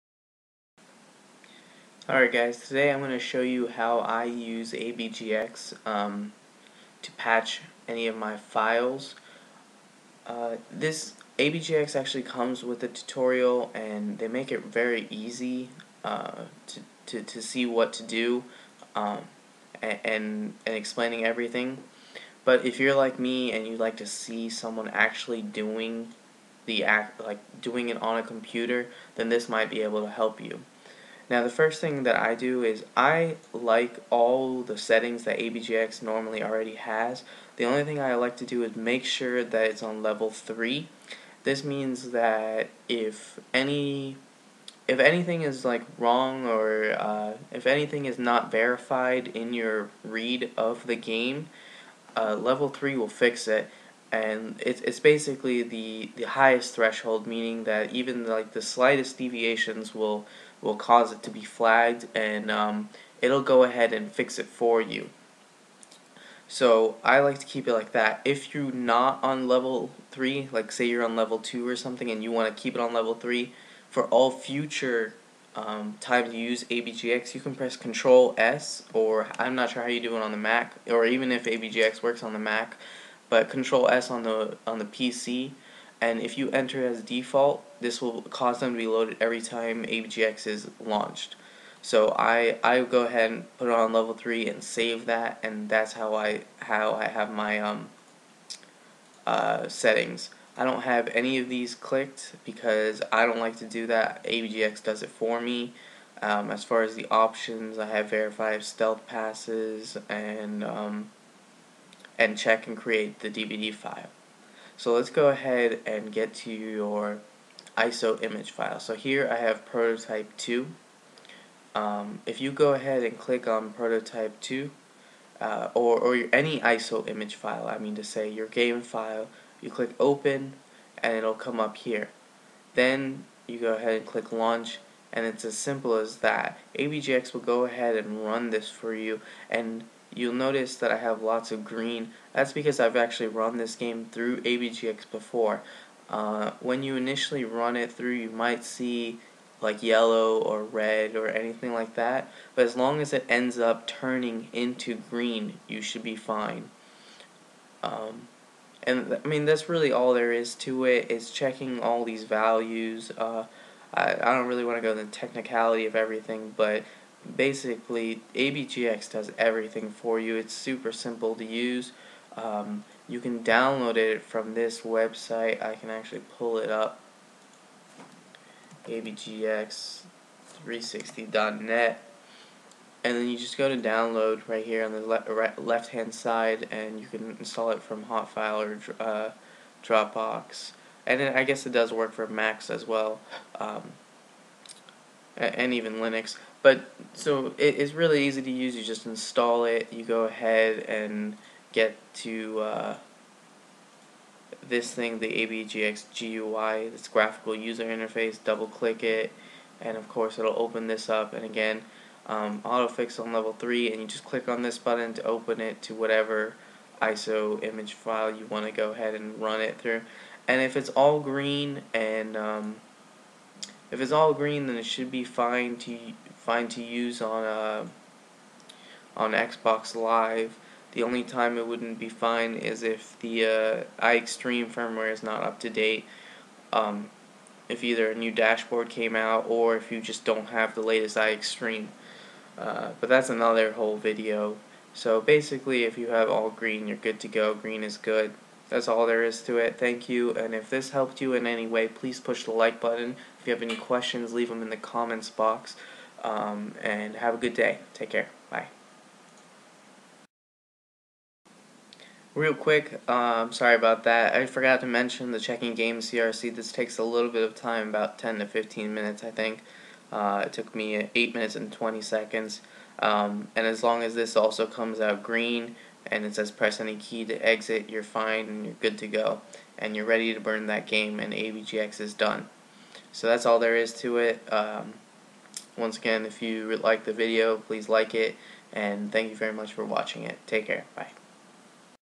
How to use ABGX correctly (Stealth Patching Games) ABGX360
Download is at agbx360.net